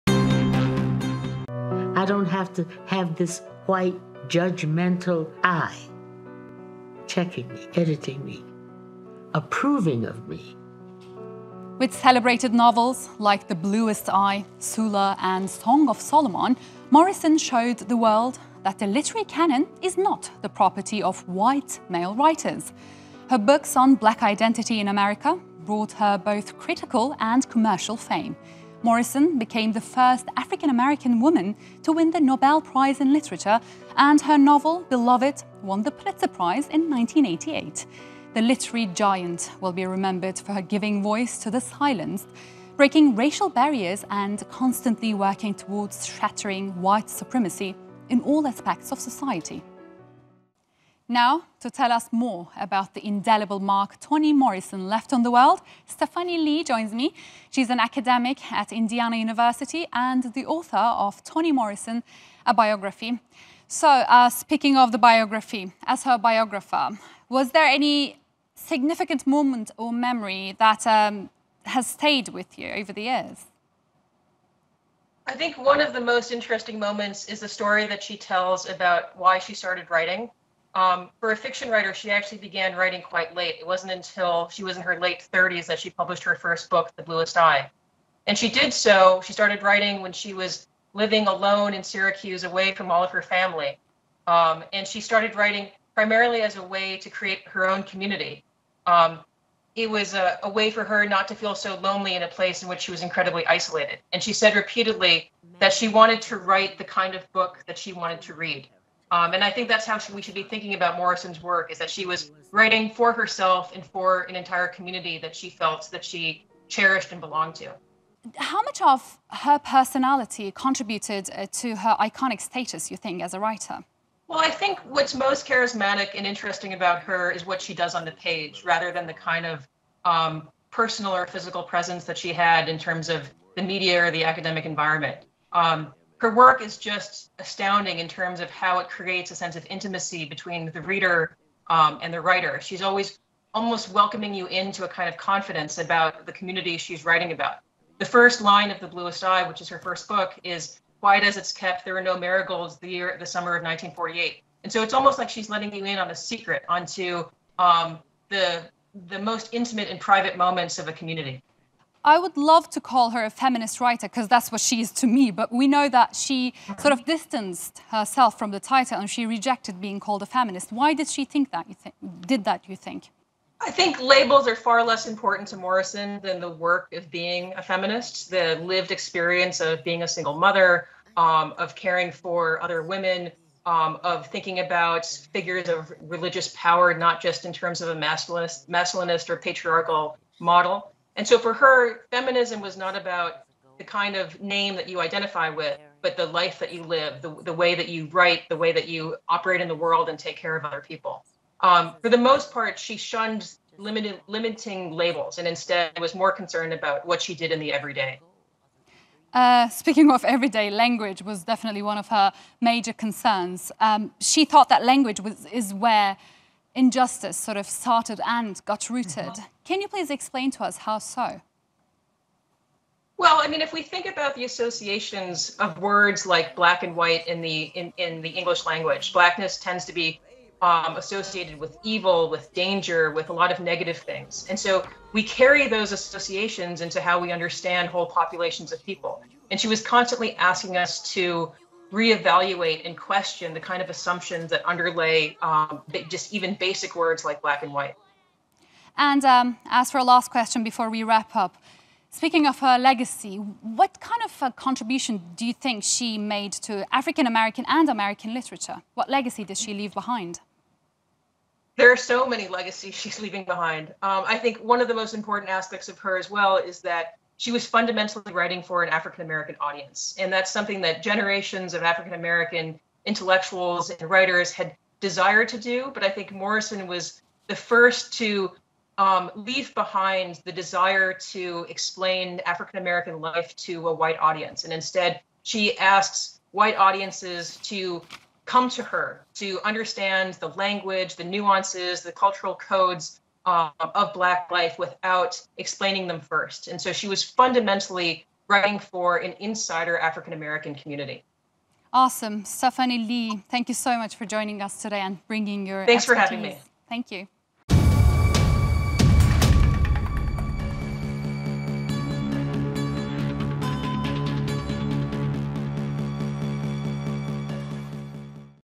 Remembering Toni Morrison
"We die. That may be the meaning of life. But we do language. That may be the measure of our lives." The author of these words, Toni Morrison, has passed away at the age of 88. The Nobel prize-winning novelist, essayist and editor was one of the most iconic writers of her time, leaving behind an immortal legacy we should all carry forward.

Stephanie Li, Susan D. Gubar Chair in Literature at Indiana University 00:51